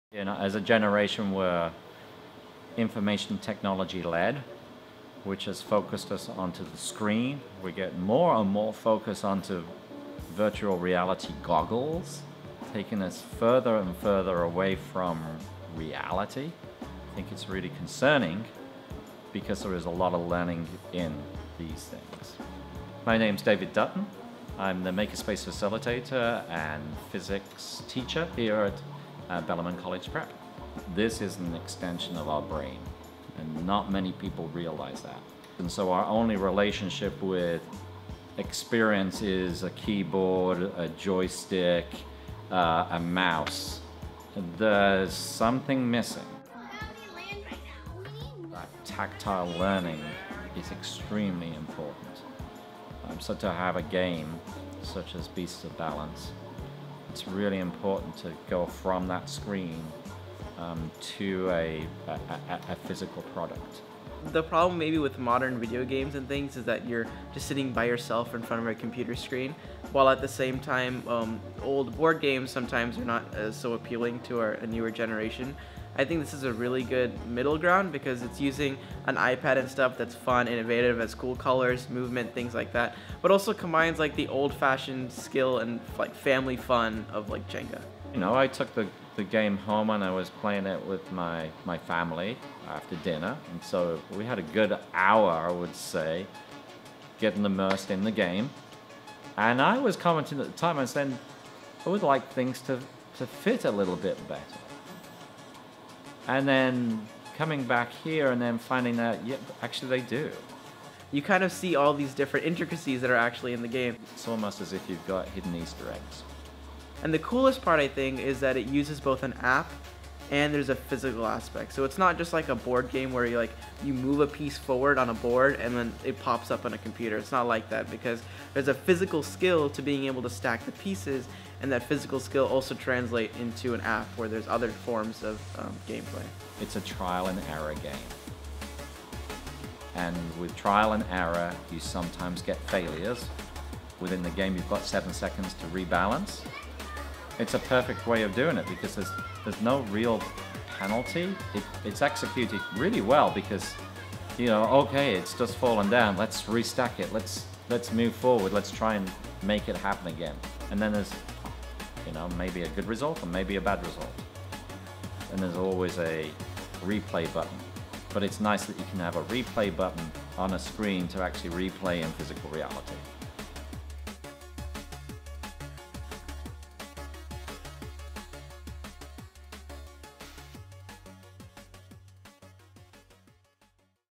Makerspace: Beasts of Balance in the Classroom
Recently we put together a blog post that shared the experiences of various educators who have used Beasts of Balance. Now, to accompany that, here's our video with Makerspace Facilitator and Physics teacher David Dutton and his thoughts on negotiating digital technology and using Beasts of Balance in the classroom.

Beasts of Balance is a stacking game of wild creation!